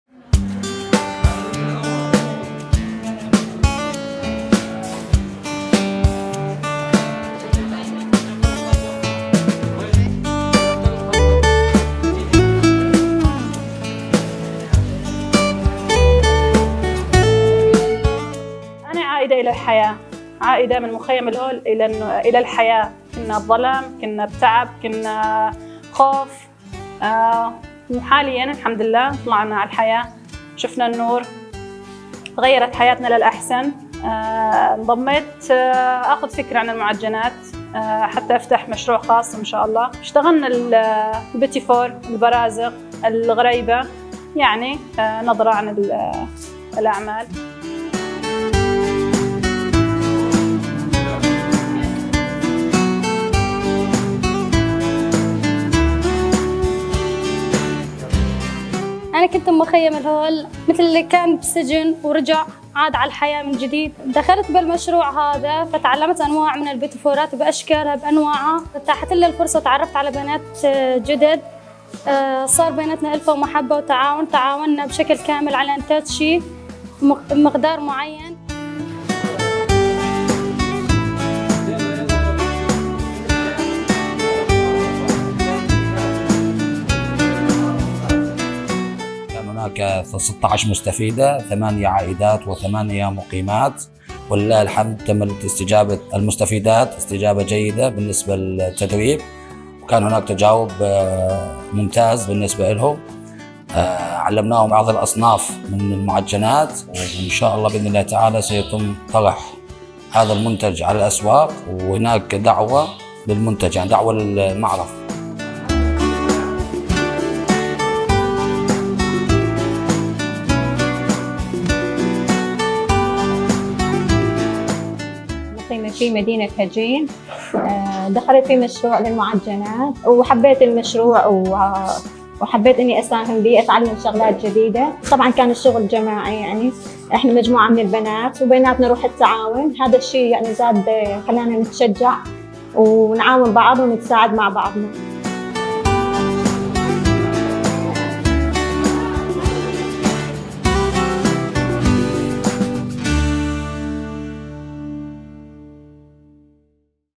"لن نكون عبئاً".. عائدات للحياة يستعدن لدخول سوق العمل بريف دير الزور
🎥 زينب طريف